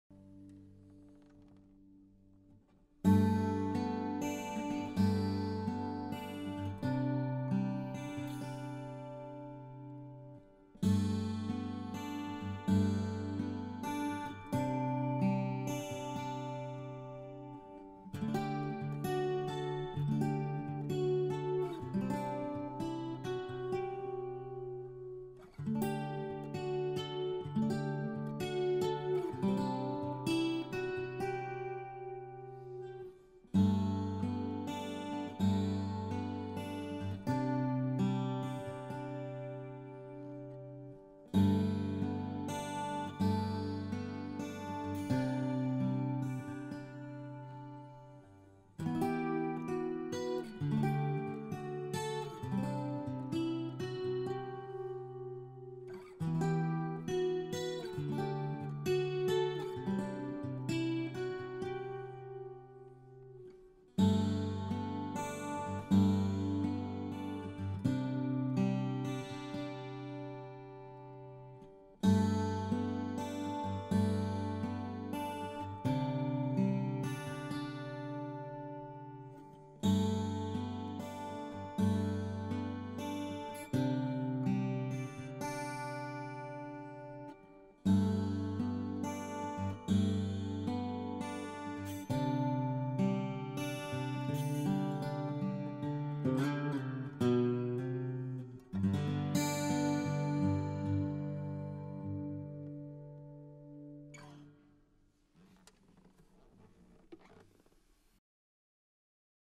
Yamaha FG Transacoustic Acoustic Guitar
Yamaha presenta la nueva serie de guitarras acústicas (con previo pasivo) de cuerpo, modelo FG. Disponibles en Hipermusic a partir de Marzo.